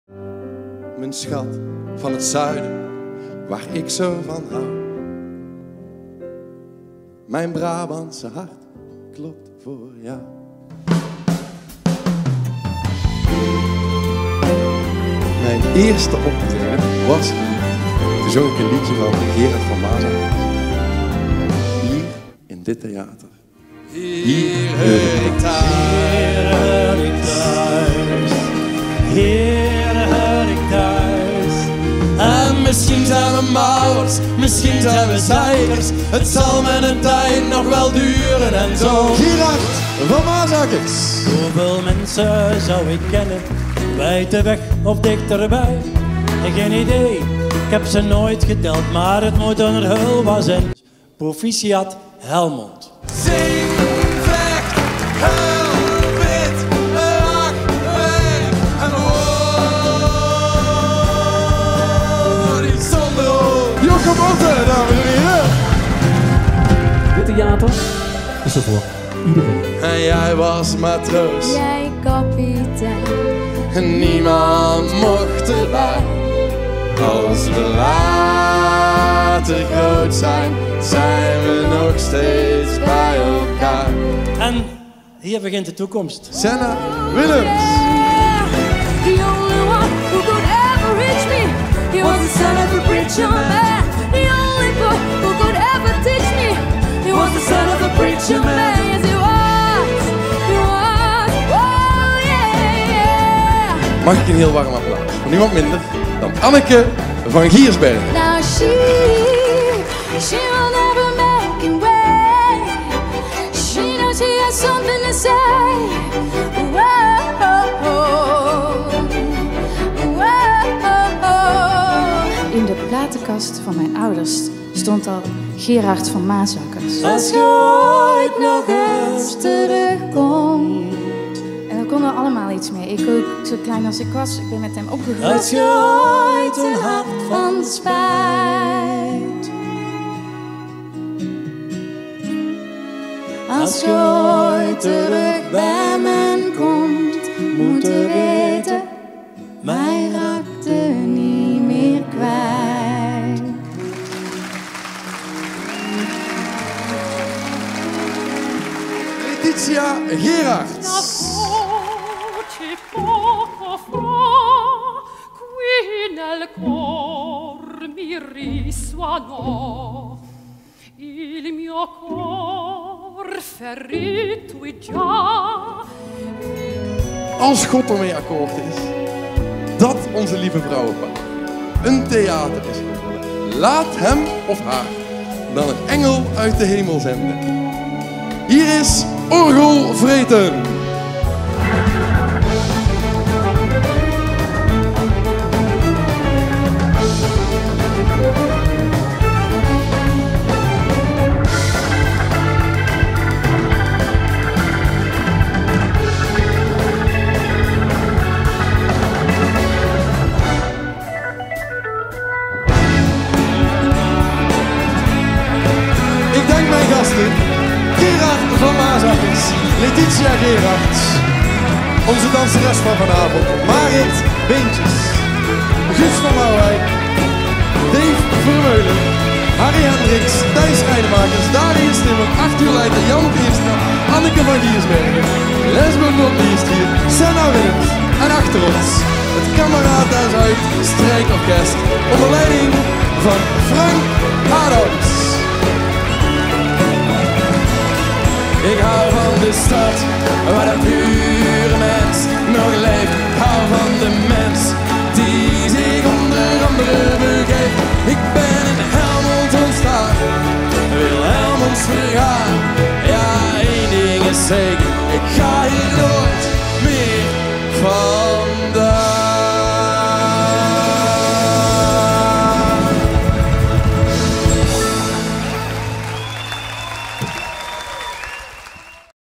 OPENINGSCONCERT 2018 / Trailer
Officiële opening van Het Speelhuis te Helmond op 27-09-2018

Onder begeleiding van: Thomas Pieters
Geproduceerd door: Frank Adams, Het Speelhuis, Thomas Pieters
Regie: Tim Goditiabois

Artiesten: Gerard van Maasakkers, Laetitia Gerards, Anneke van Giersbergen, Senna Willems, Orgel Vreten

Orkest: Kamerata Zuid
Dirigent: Frank Adams
Zakelijk leider orkest: Esther Coppens – Kemmere

Arrangementen orkest: Ruud Peeters, Ron Antens

Bas: Jan Teerstra
Drum: Arthur Lijten
Gitaar: Harry Hendriks
Toetsen: Guus van Marwijk
Hammond: Thijs Schrijnemakers, Darius Timmer
Special guest/elektrische gitaar: Dave Vermeulen

Producent film: Deelen Tekst en Beeld
Camera: Rob Deelen, Mike Versteegh, Martijn van Disbergen
Live Geluid: Jonathan Heywood
Audiomixage: Gabriël Peeters

Lichtontwerp & techniek: Theo Pluijm, Wim van Bokhoven, Wim Jansen, Rick Molenaar, Veenerick Demmers

Jochem Otten
Alle medewerkers het Speelhuis
Mogelijk gemaakt door Gemeente Helmond